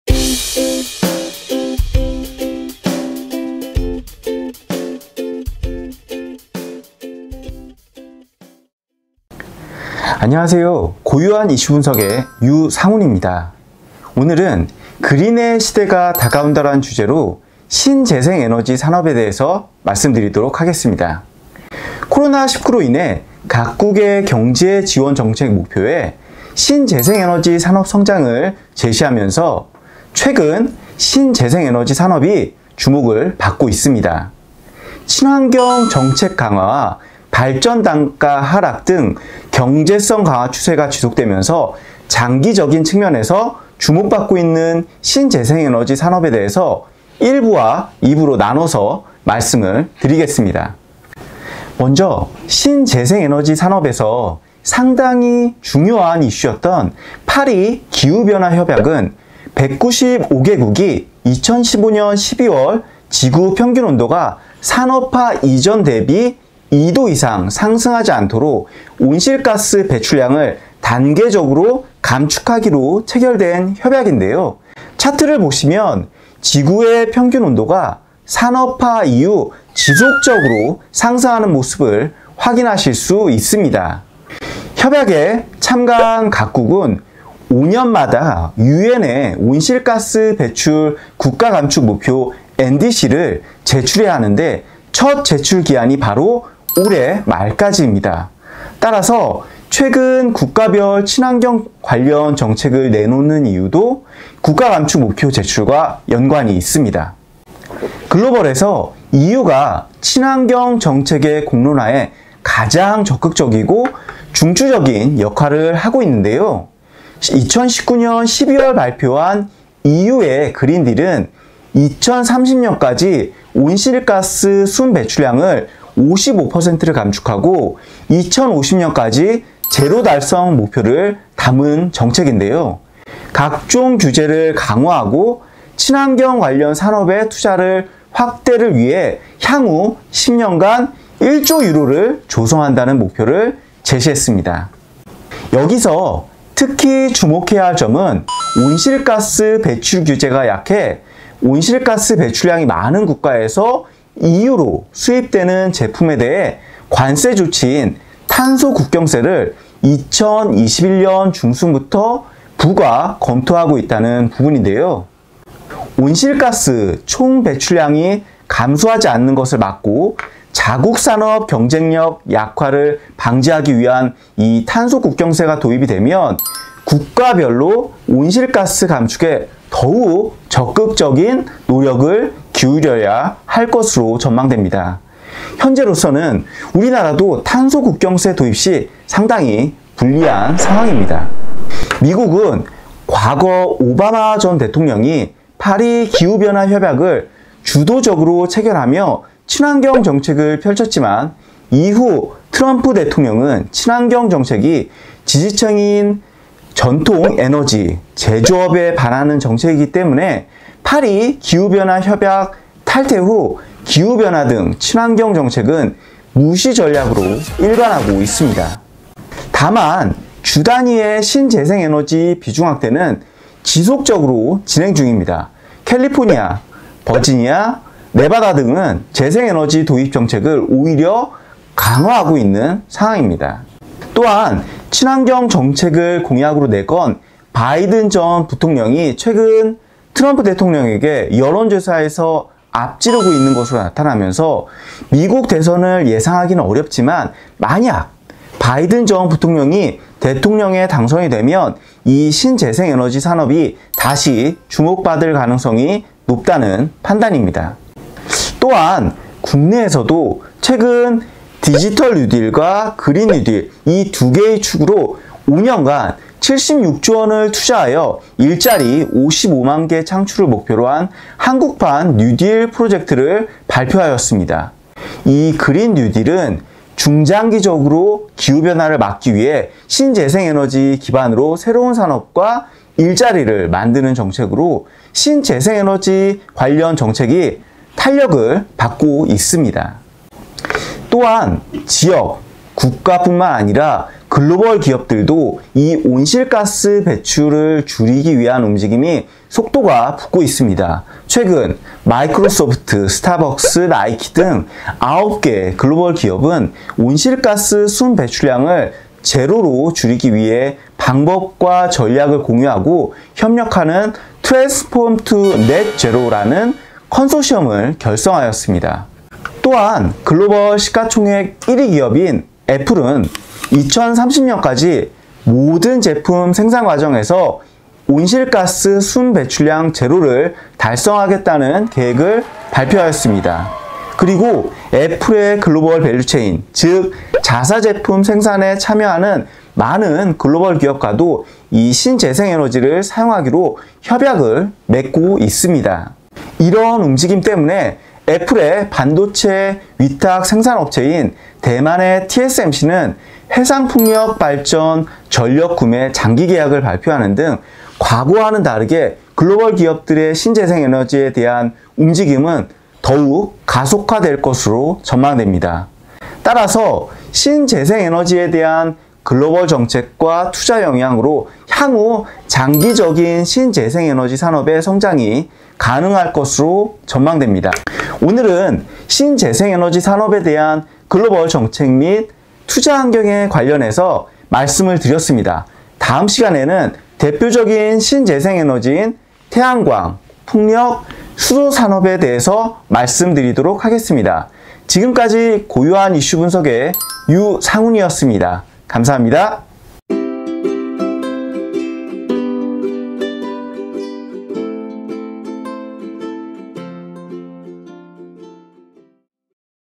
[高有한 이슈분석] #4. 그린(Green)의 시대가 다가온다. Part 1.
[高有한 이슈분석] #4. 동영상입니다.

"그린(Green)의 시대가 다가온다"라는 제목으로
Part 1과 Part 2로 나누어 업로드 예정입니다.

Part 1 영상에서는 최근 신재생에너지 산업이 주목받는 이유와
신재생에너지 산업 진행 현황에 대해 설명합니다.